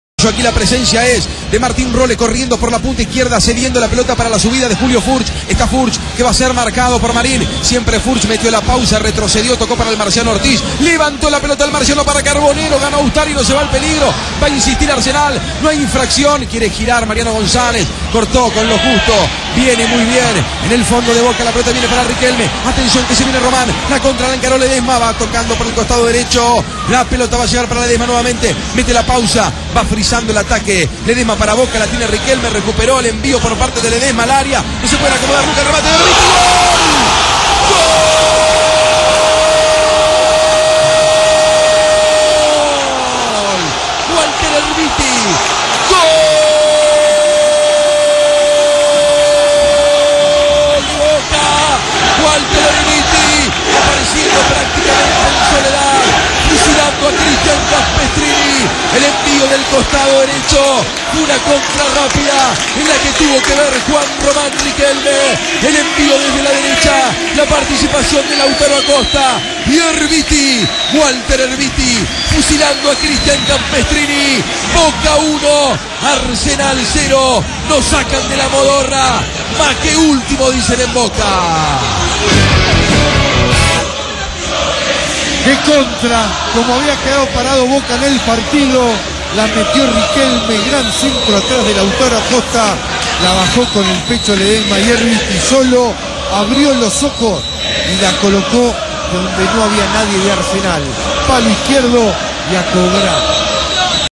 Boca 1 Arsenal 0 (Relato Juan Pablo Marron) Torneo Final 2013 (16/6/2013)
El gol del partido con relato de Juan Pablo Marron Fm Rock & Pop 95.9